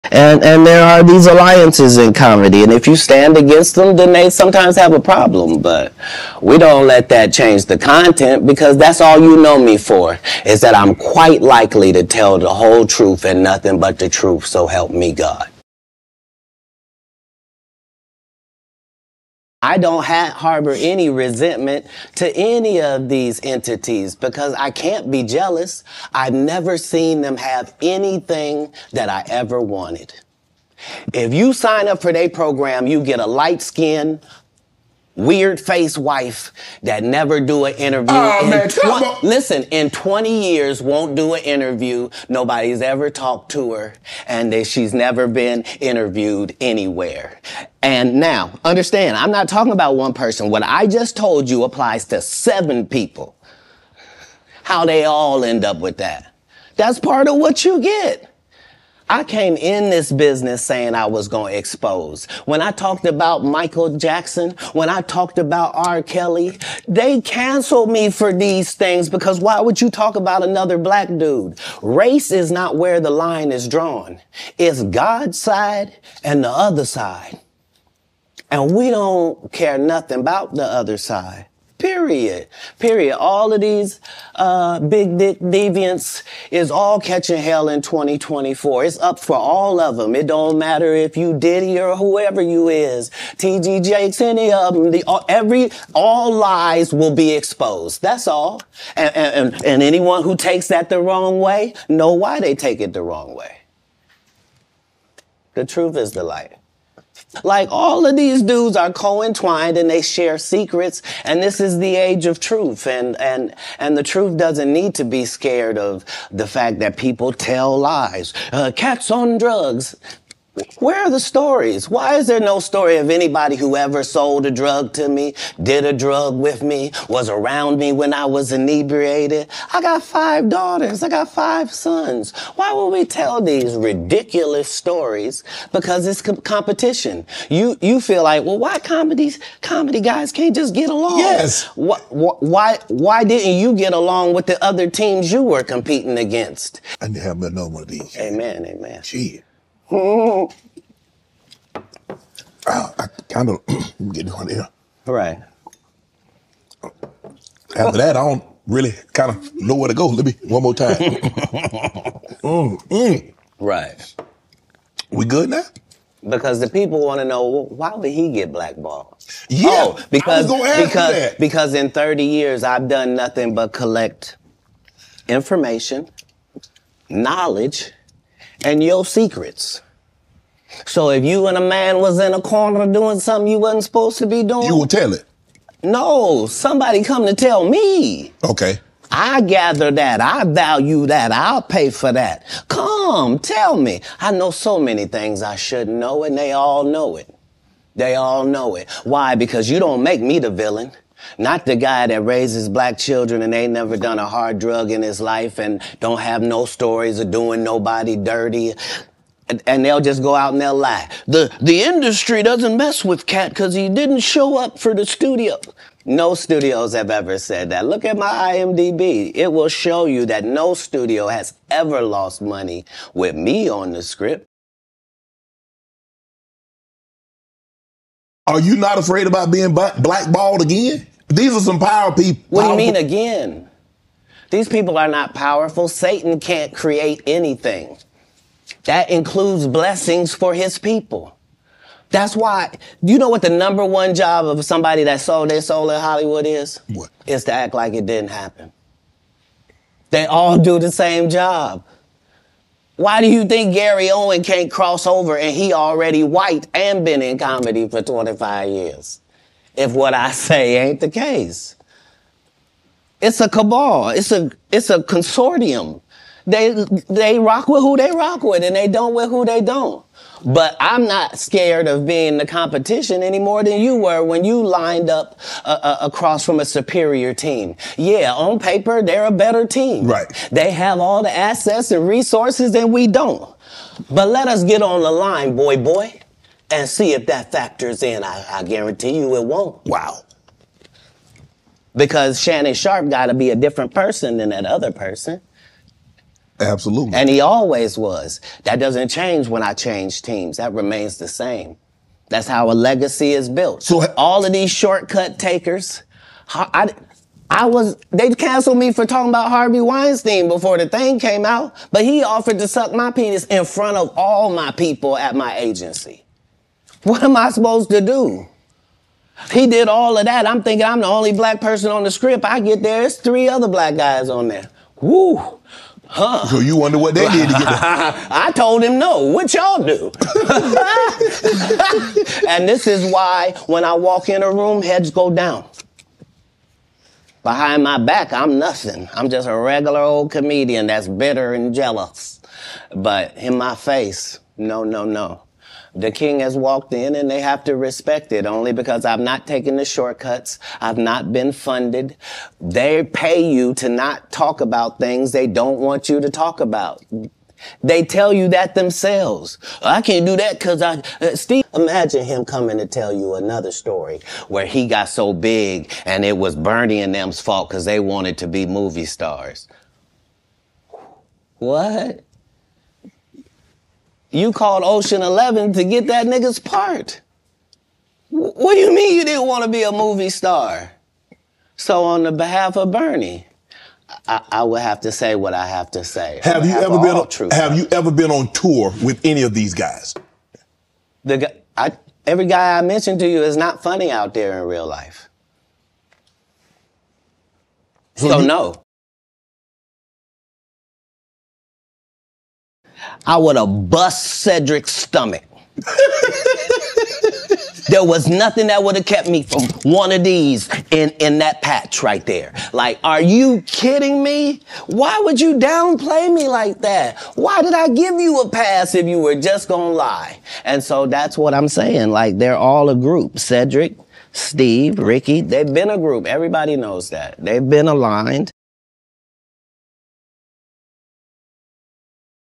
KATT WILLIAMS EXPOSES HOLLYWOOD🌜🍆
Katt Williams had my mouth on the damn floor. I can't believe he spilled so much tea. I'm going to be looking at everyone with the peoples eyebrow and a cricked neck. What are your thoughts? I really want to know how you feel about everything in the comment section below.

Time stamps
Michael Jackson 1:04
R. Kelly 1:05
God 1:15
Celebrities Getting Exposed 1:28 Diddy T.D. Jakes
Secrets 1:51
Katt Williams Drugs Allegations 2:00
Shannon Sharpe Takes A Drink 2:30
Katt Williams Collecting Dirt 2:55-3:20
Shannon Addresses Future Blackball 3:50
Illuminati 4:05
Gary Owens 4:15
Harvey Weinstein 5:10-5:30 Dick Sucking Allegations
Not Funny Comedians 6:33
Cedric The Entertainer Threat 6:44